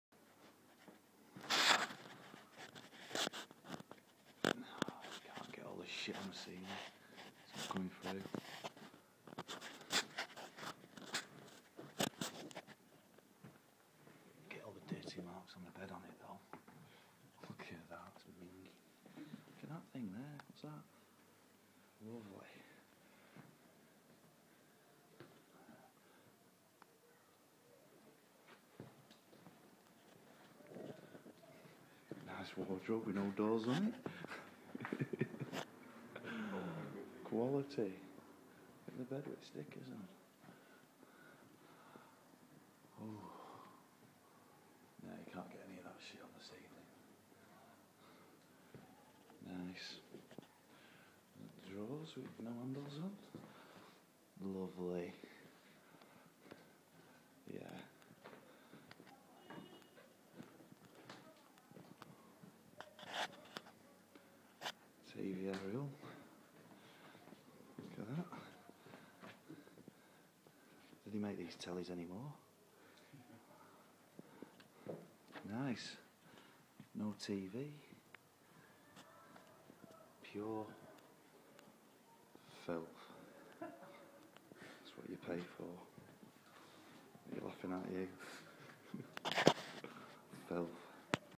The dirtiest hotel in Wolverhampton
Can't beleive how minging this hotel really is- pure filth